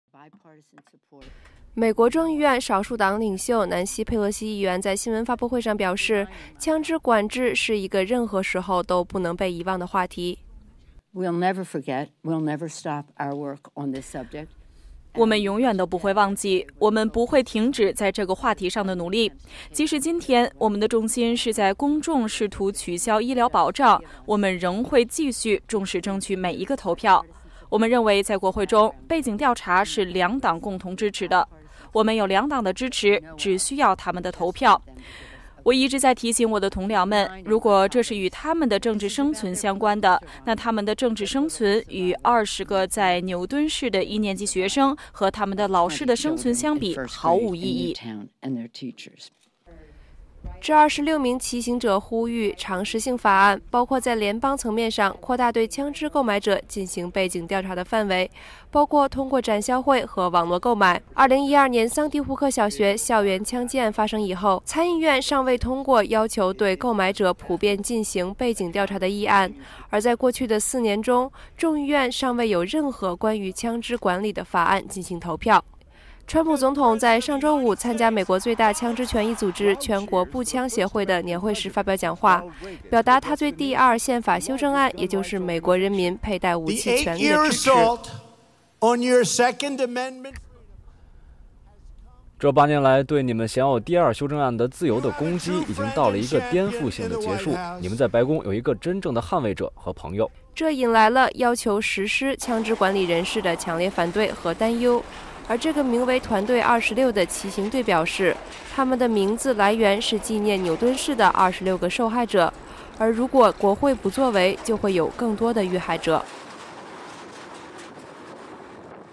自行车骑行400英里 缅怀桑迪·胡克枪击受难者
为缅怀2012年桑迪·胡克小学校园枪击案的受难者，一个骑行志愿者团队，周四从华盛顿出发，前往枪击案发生地康涅狄格州，以呼吁更严格的枪支管理法案，连线美国之音记者尼亚做更多报道。